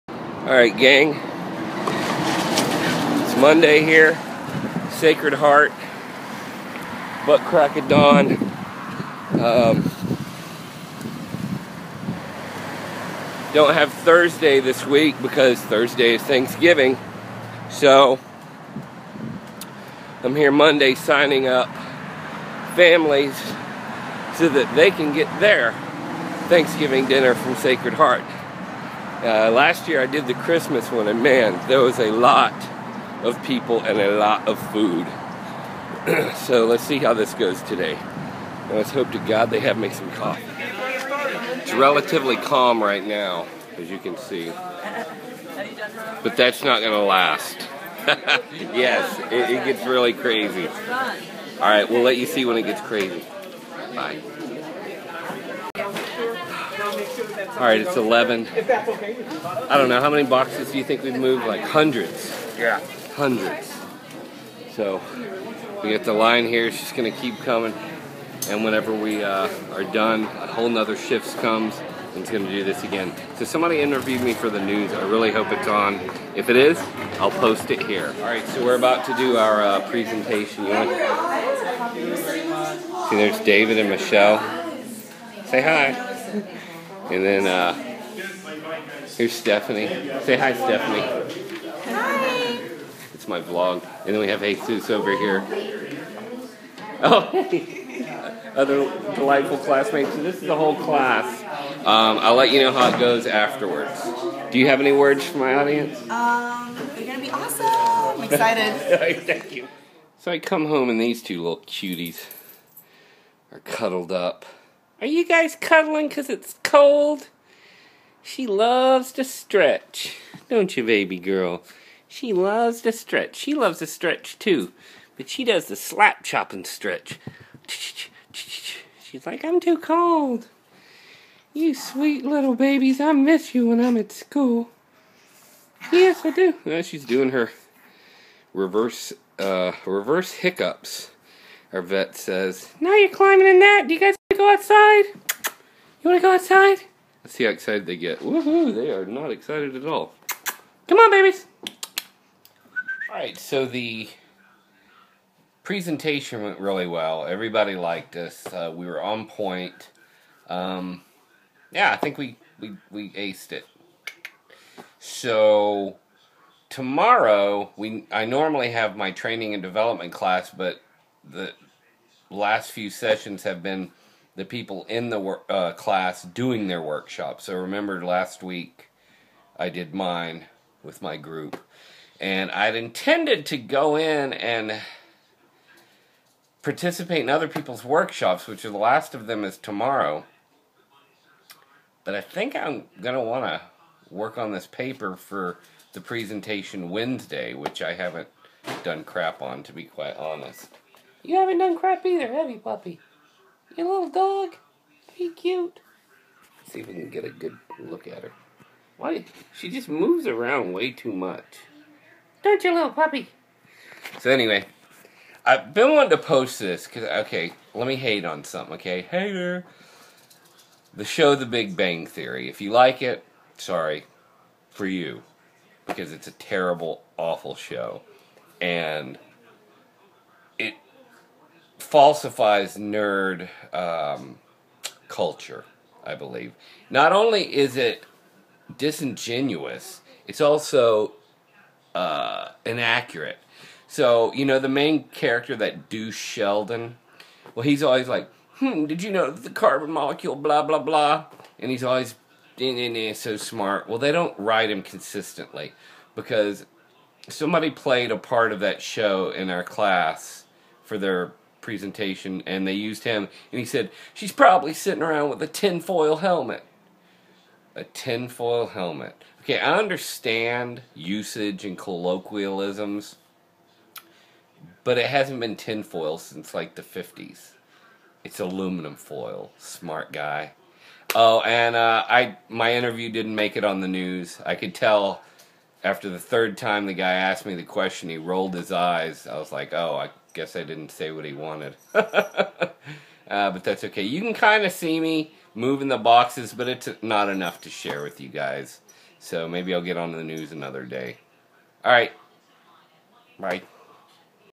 11-25-13
Sacred Heart holiday meal give away, Comm 150 presentation and puppies!